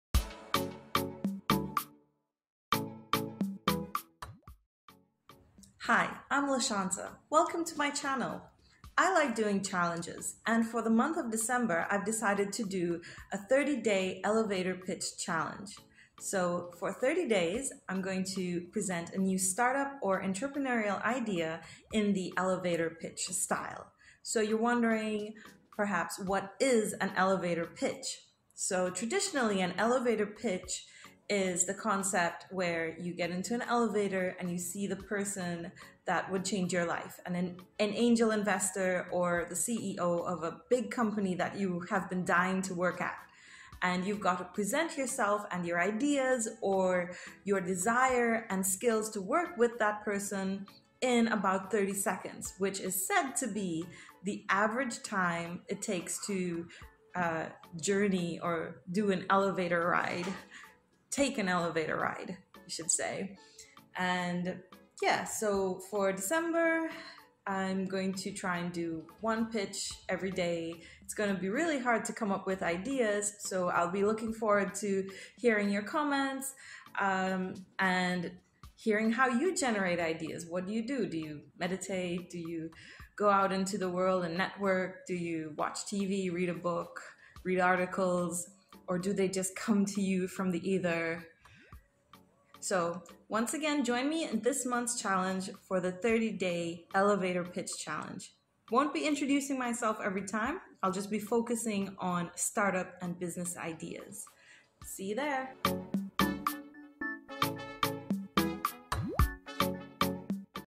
Intro to December challenge
Hi everyone, I was gifted the idea of doing this unique challenge - a pitch a day for 30 days! This December I will attempt to upload a new elevator pitch with a new idea for a startup or business for 30 days straight! It all starts on the 1st!